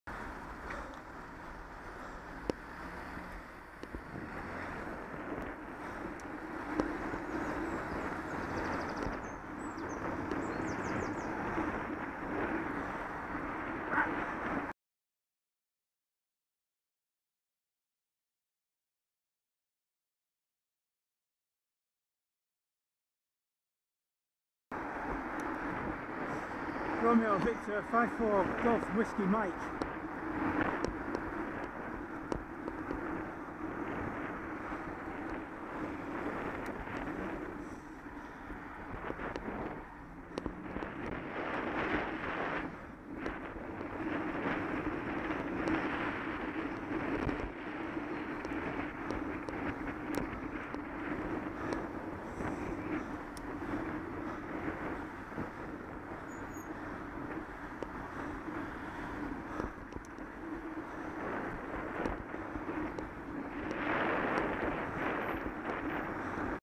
Avoiding Bolton Council's repainted suicide lane.
Someone filmed me on his phone avoiding the suicide lane that Bolton Council has repainted after resurfacing work. A wasted opportunity to put in some decent cycle infrastructure.
I do hope he posts the video on YouTube. There was a bus following me that would almost certainly have shown one of the reasons why this cycle lane is dangerous.